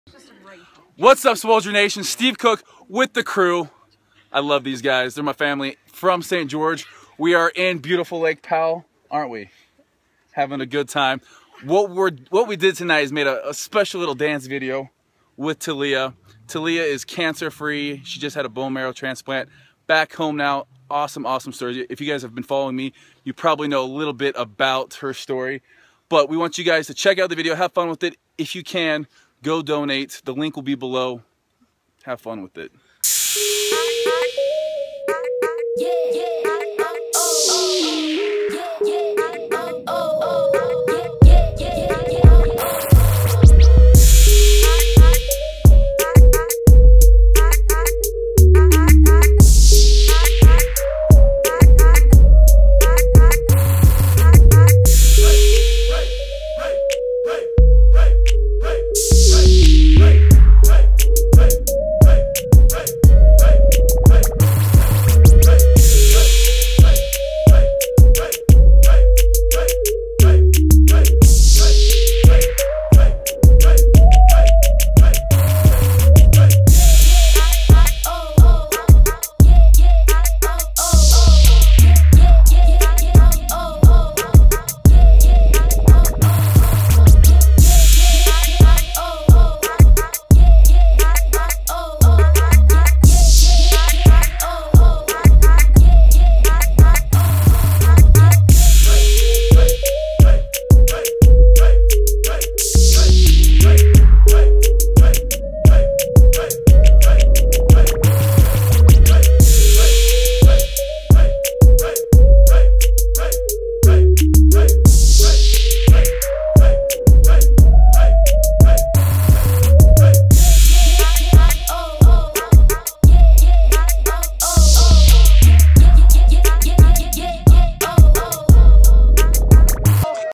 Cancer free Dancin!!!
Taleah is Home after battling Acute Lymphoblastic Leukemia for over three years!!